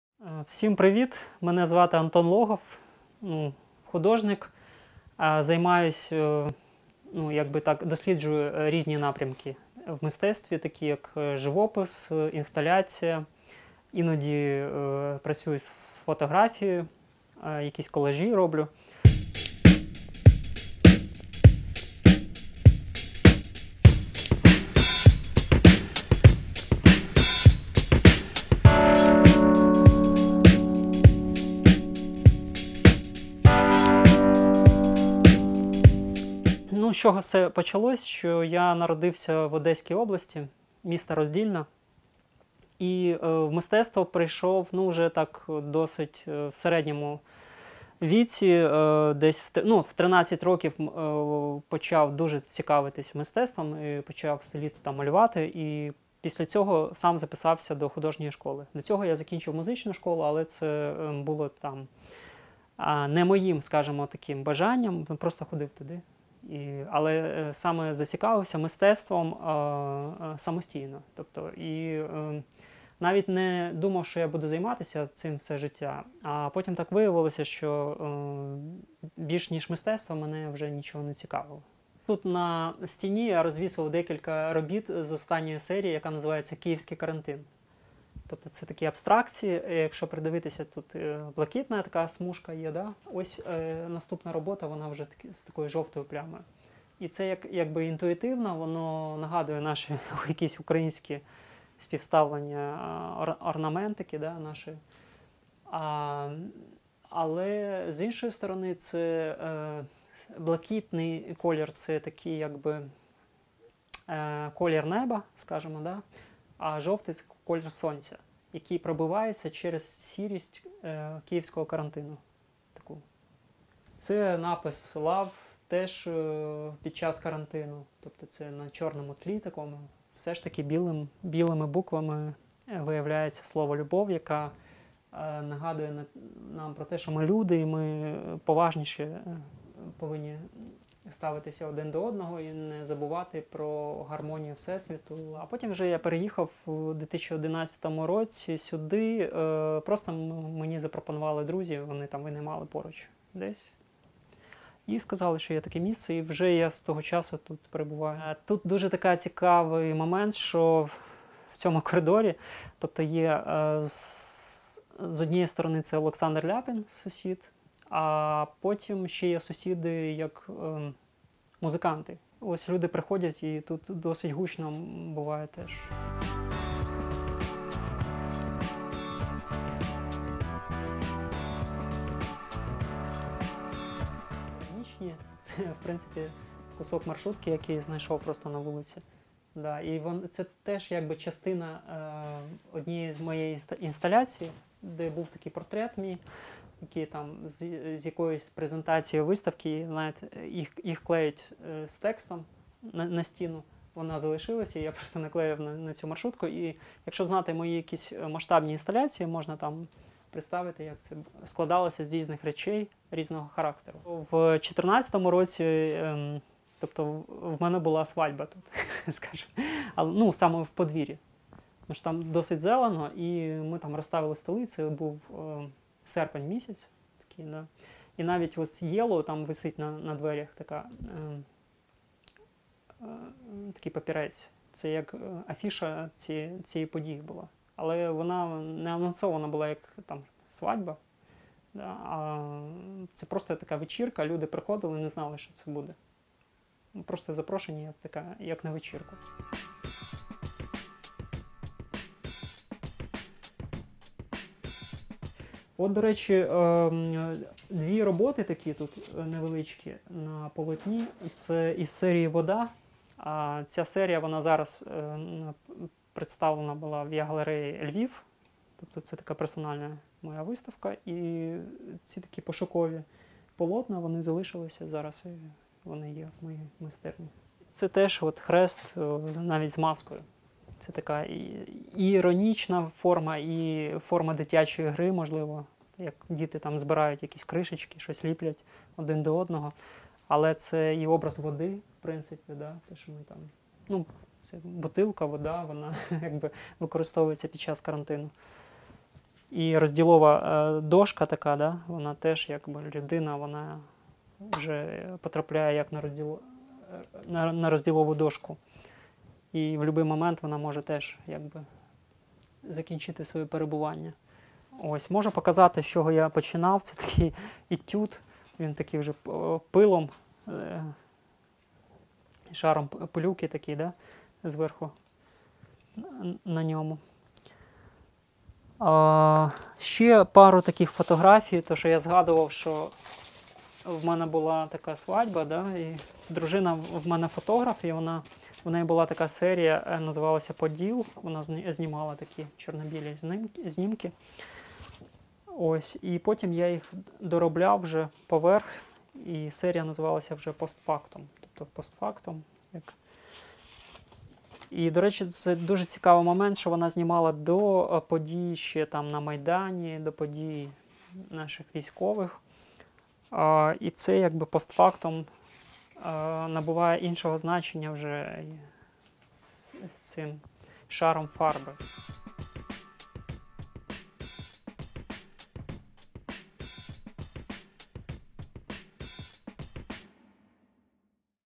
В майстерні художника | Антон Логов
Нова рубрика на нашому каналі "В майстерні художника".

В цій рубриці ми, разом з арт-центром Я Галерея, приходимо в гості до відомих митців та показуємо інсайти з їхніх майстерень.

Сьогодні ми в гостях у Антона Логова.